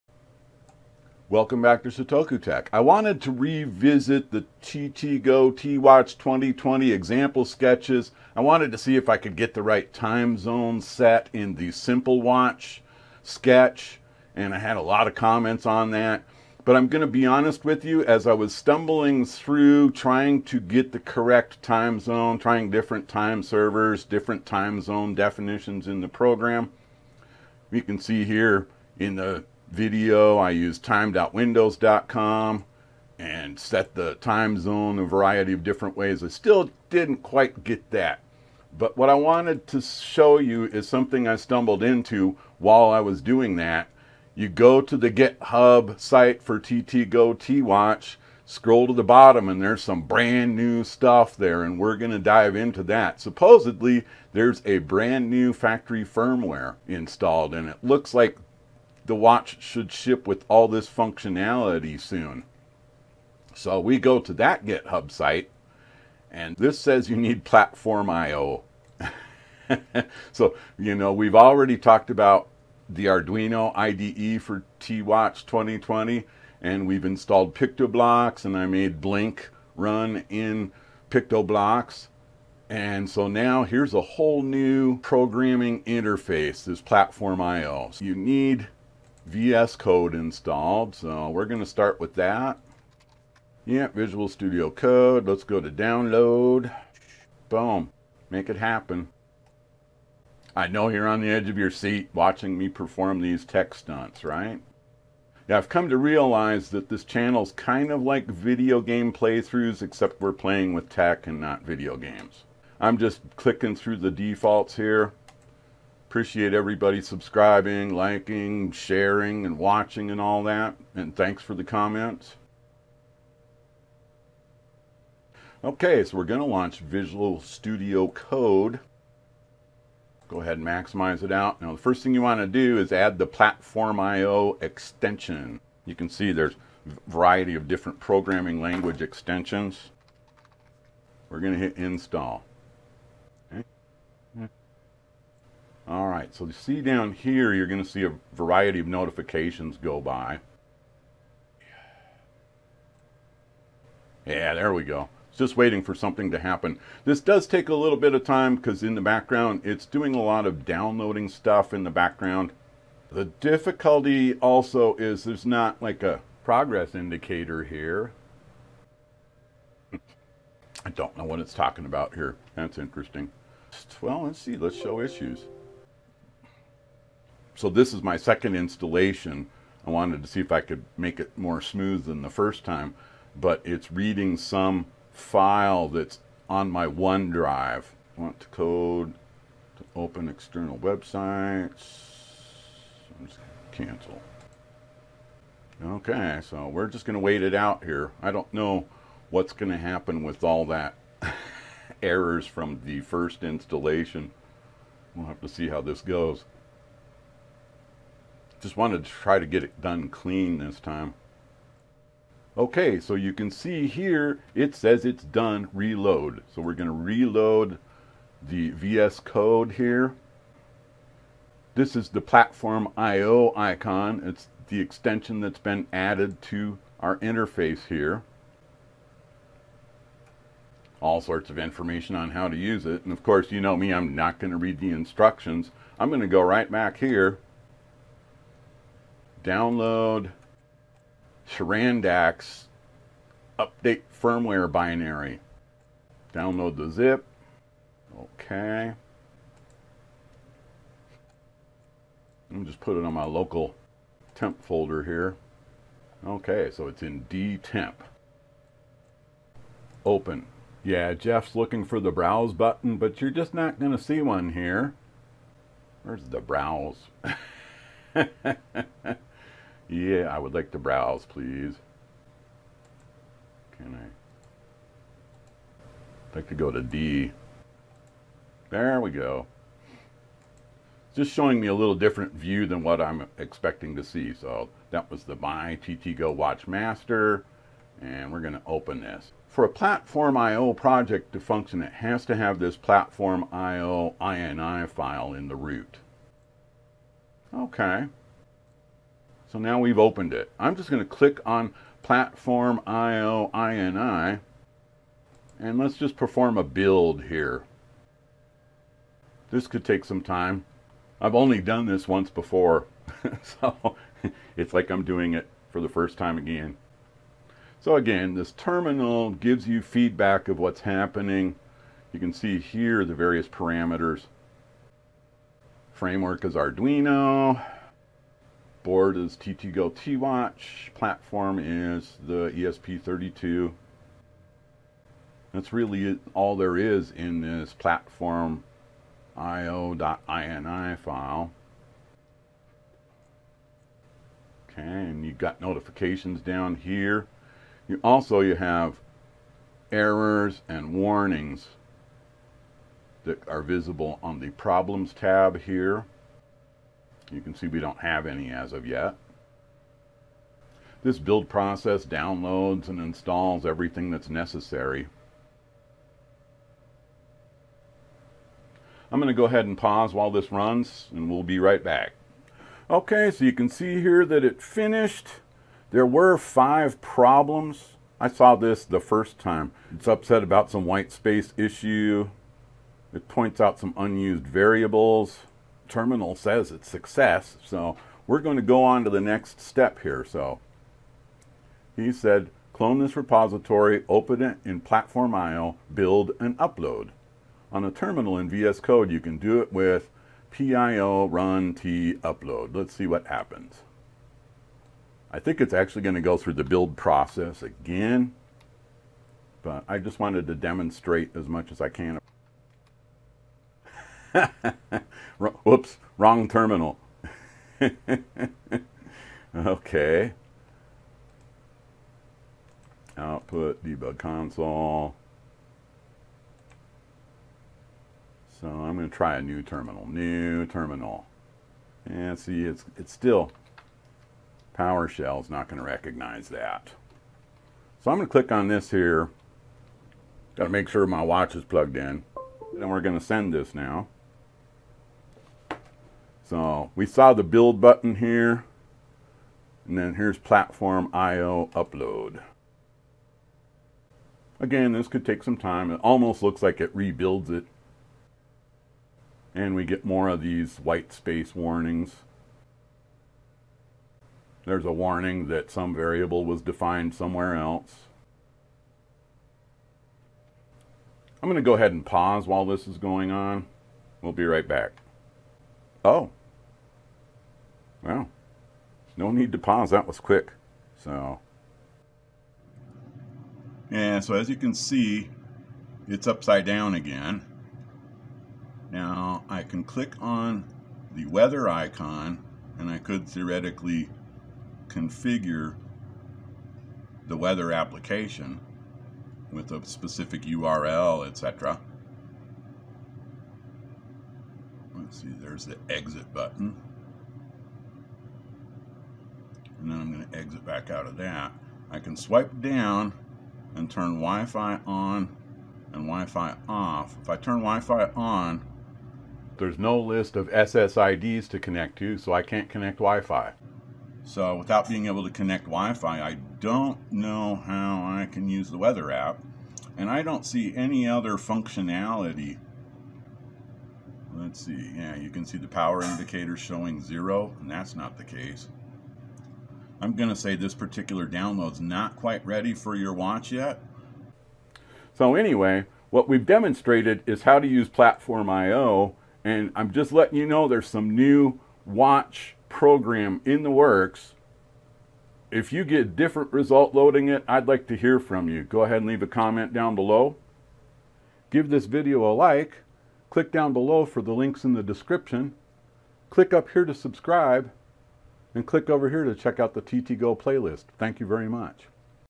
TTGO T-Watch 2020 New Firmware in PlatformIO
I was going to make another video addressing comments from the various TTGO T-Watch 2020 sketches around time zones and time setting. But I stumble instead into this new TTGO T-Watch 2020 firmware update. Of course they use PlatformIO in Visual Studio Core to apply it. So here we go down the road of another programming interface. Watch to see how it turns out!